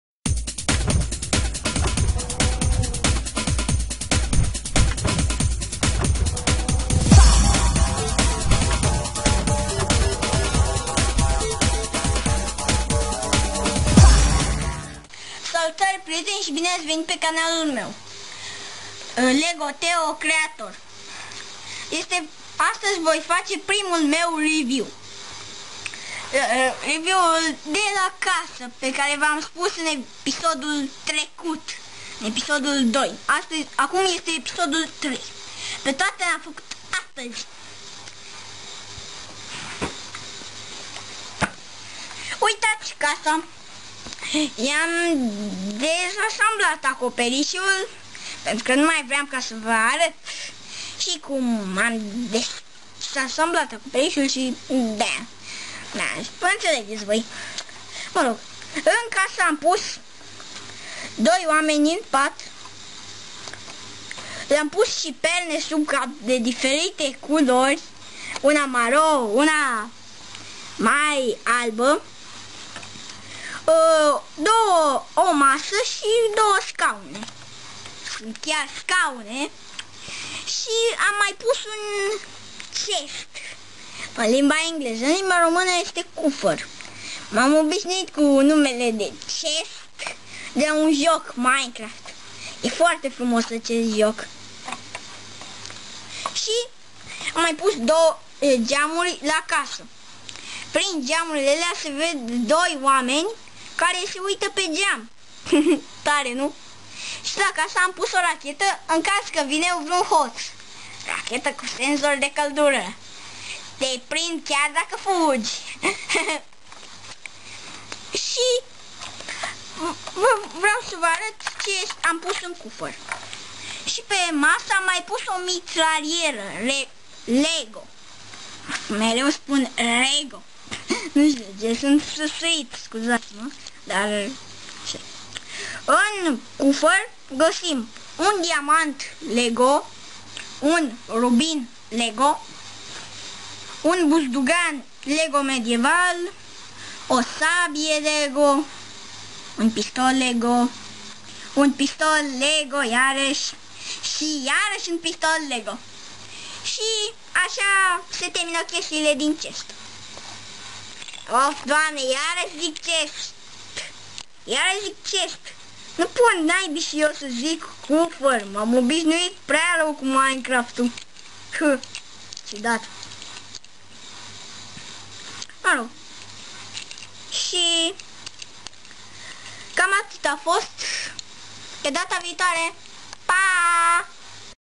Review la o casa din piese LEGO
Va voi arata cum sa faceti aceasta casa cand voi atinge numarul de 100 de abonati!!

    ★Va doresc o zi frumoasa in continuare♥

       ★Have a nice day further♥